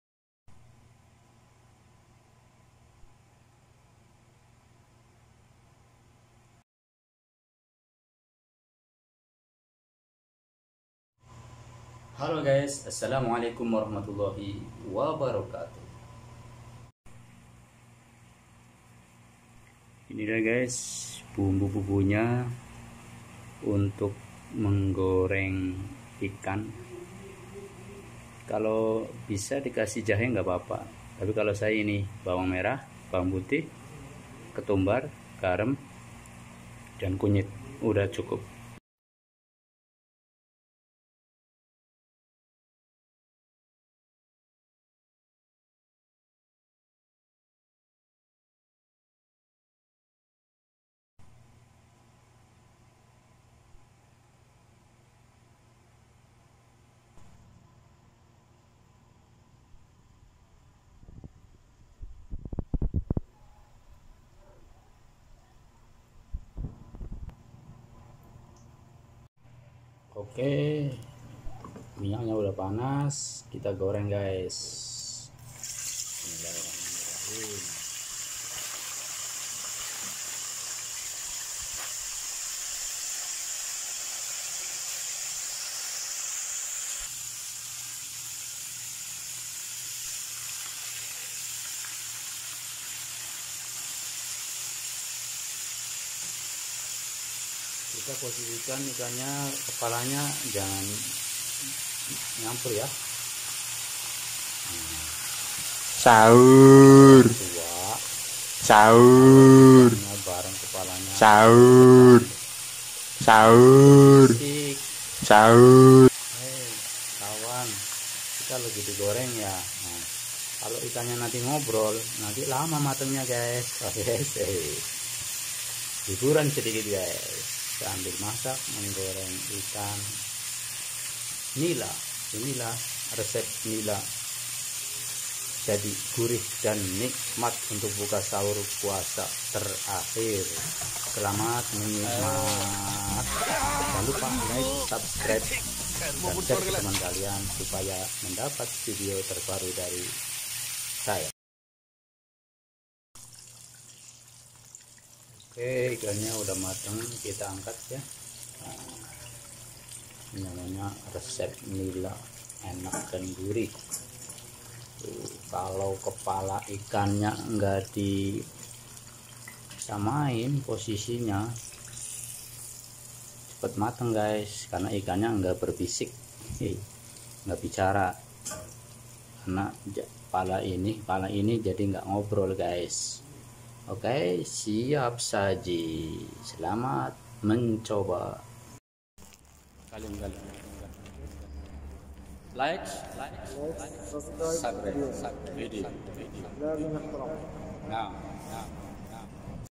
Rahasia Goreng ikan rasa gurih dan nikmat buat sahur
Salkomsel dan selamat ied lahir batin juga  mohon maaf ada kesalahan teknis sedikit rahasia goreng ikan rasa gurih dan nikmat
terima kasih yg support channelini jangan lupalikedansubscribe juga share ketemen